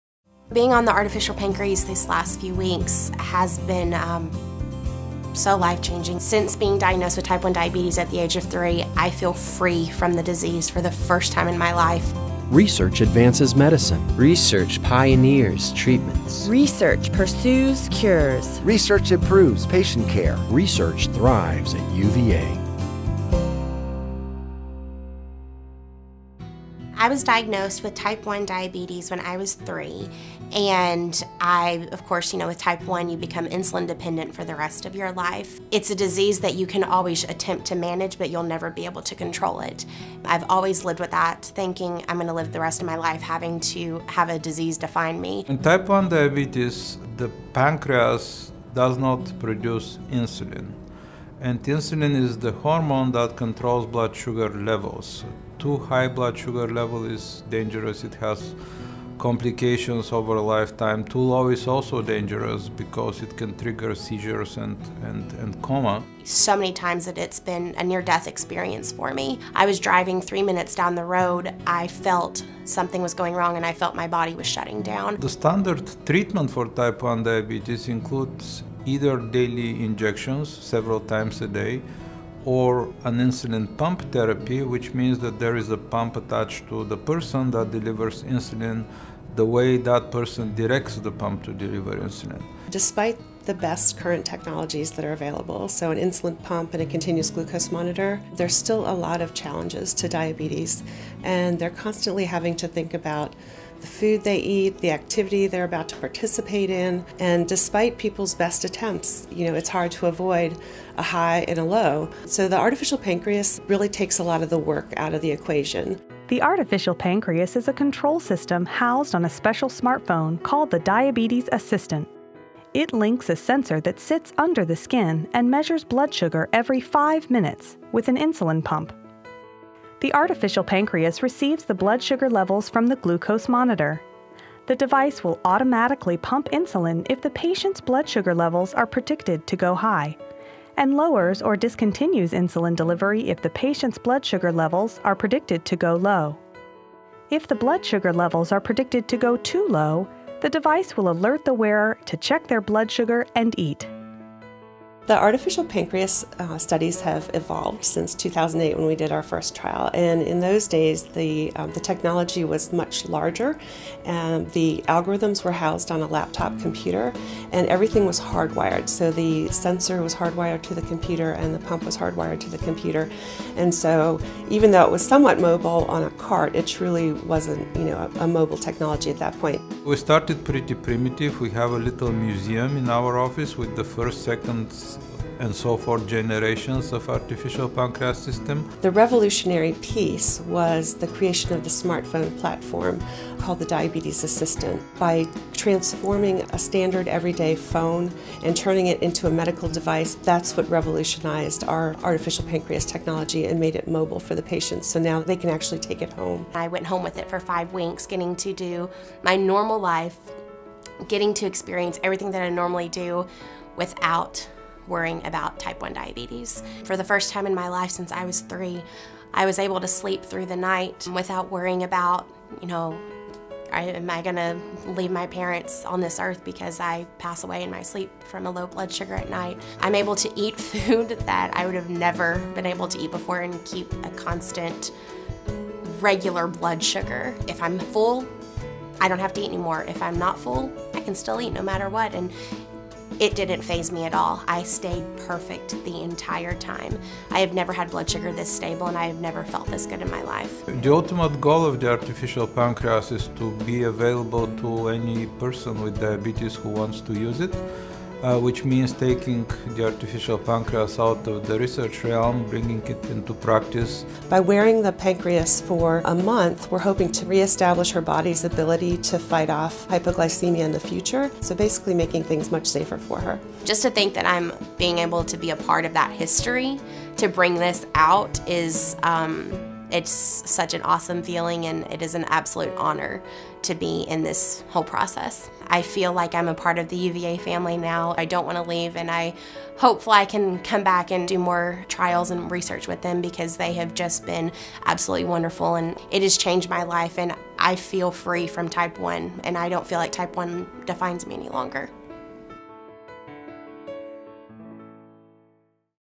UVA Research: The Artificial Pancreas App
The artificial pancreas app uses an algorithm to automatically deliver the proper amount of insulin and helps to control blood sugar levels. The technology has been developed by researchers at University of Virginia.